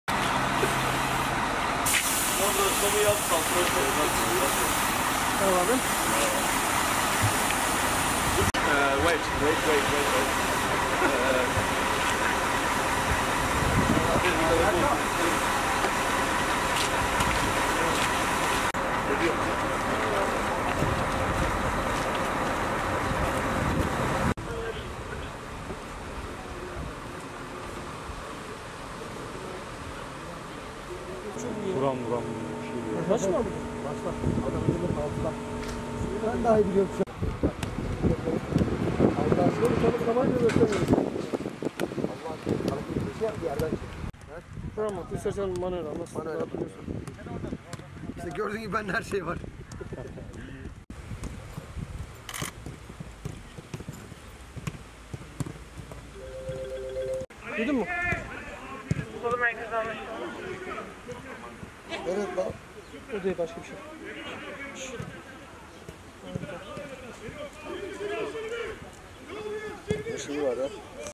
MKE Ankaragücü, Bolu’da yeni sezon hazırlıklarını sürdürdü
MKE Ankaragücü, Bolu kampında yeni sezon hazırlıklarını, akşam saatlerinde yaptığı antrenmanla sürdürdü.